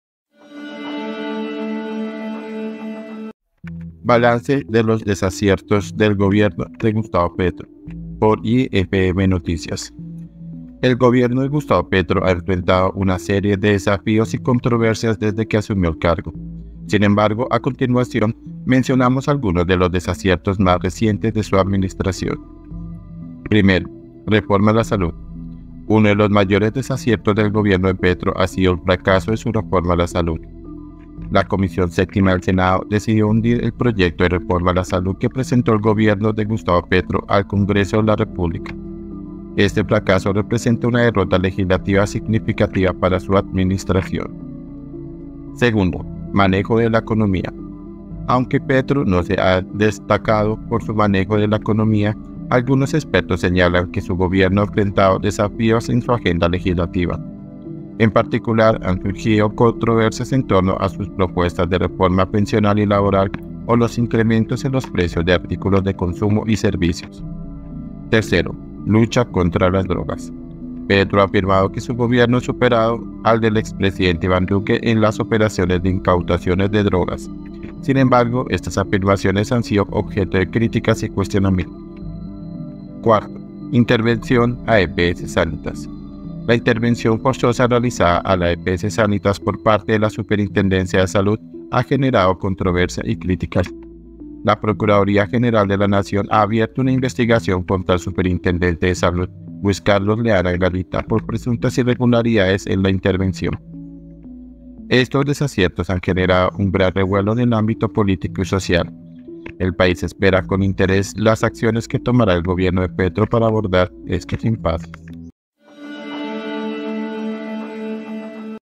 BALANCE DE LOS DESACIERTOS DEL GOBIERNO PETRO 🔵 Por IFM NOTICIAS
El gobierno de Gustavo Petro ha enfrentado una serie de desafíos y controversias desde que asumió el cargo. Sin embargo, a continuación, mencionamos algunos de los desaciertos más recientes de su administración:
IFM NOTICIAS